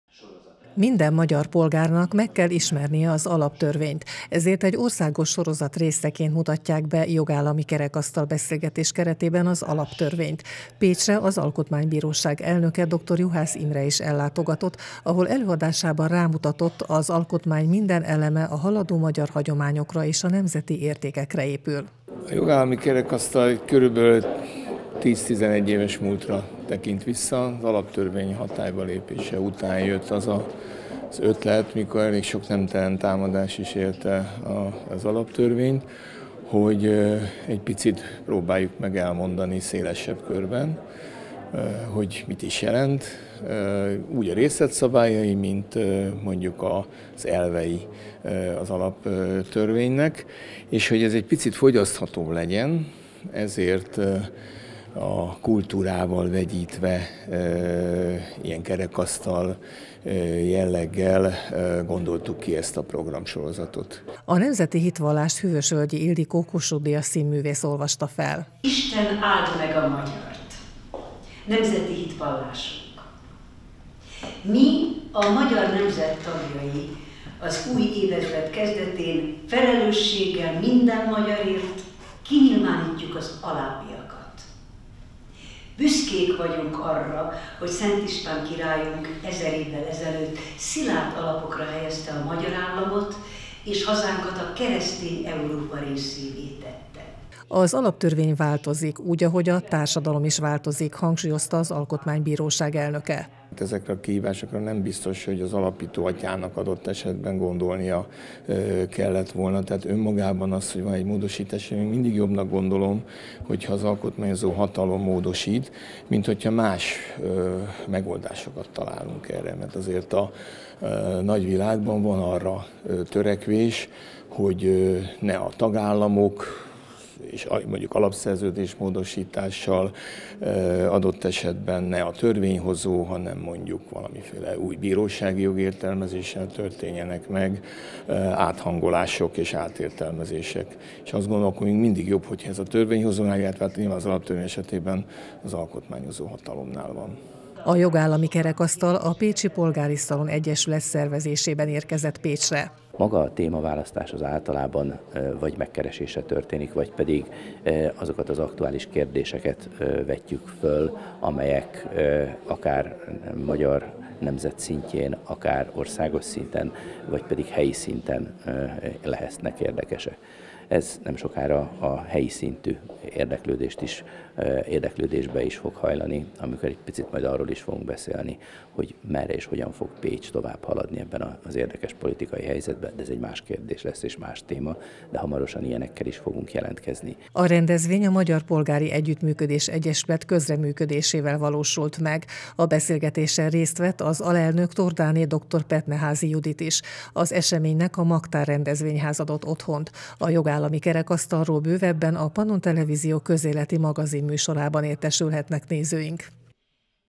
Jogállami kerekasztal - előadás az Alaptörvényről Pécsen
Jogállami kerekasztal címmel előadást szervezett a Pécsi Polgári Szalon Egyesület a Magyar Polgári Együttműködés Egyesület közreműködésével szerdán Pécsen. A rendezvény egy országos sorozat része, melynek célja megismertetni az Alaptörvényt. Az est során előadást tartott többek között Dr Juhász Imre az alkotmánybíróság elnöke. Az eseménynek a Magtár Rendezvényház adott otthont.